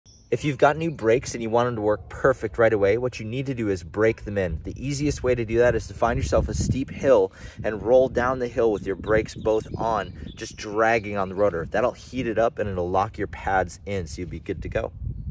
Quick Tip For Perfect Brakes! 💯
Here's a super easy technique to burn in your disc brakes to make them work perfect right away!